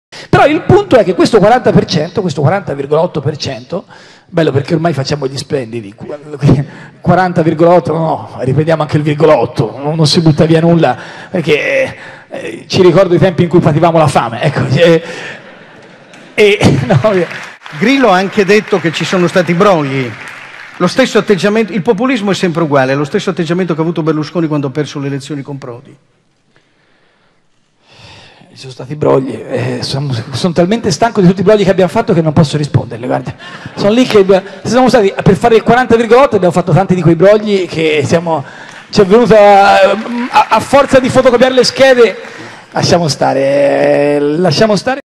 RepIdee, Renzi: "Accuse di brogli? Ne abbi...
"Ormai facciamo gli splendidi sul 40,8 perché non si butta nulla". Così scherza Matteo Renzi parlando dei risultati delle europee. Poi, incalzato da Ezio Mauro che cita l'accusa di brogli da parte di Grillo, il premier risponde con ironia: "Per arrivare al 40 per cento abbiamo fatto tanti di quei brogli..."